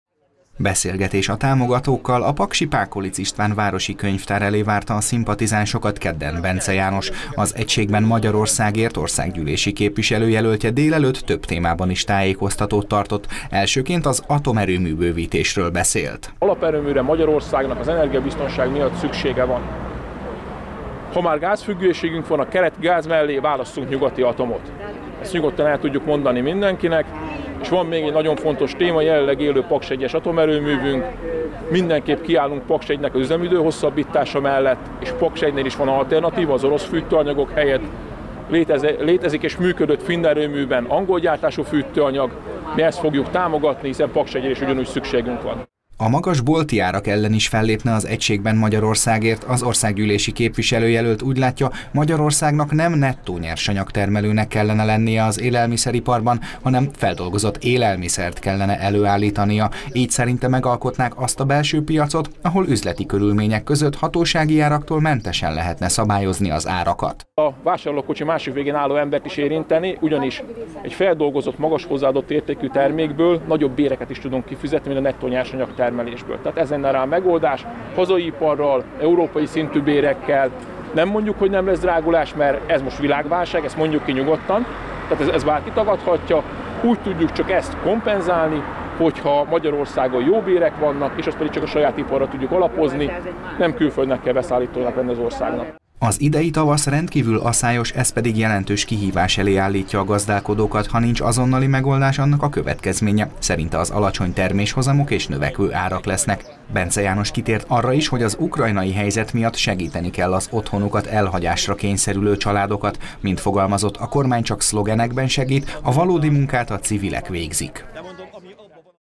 Pakson kampányolt Bencze János
Az atomerőmű bővítés jövőjéről, a magas bolti árak megfékezéséről és az ukrajnai helyzet elől menekülők segítéséről is beszélt keddi sajtótájékoztatóján Bencze János. Az Egységben Magyarországért országgyűlési képviselő-jelöltje március 22-én Pakson találkozott a támogatókkal.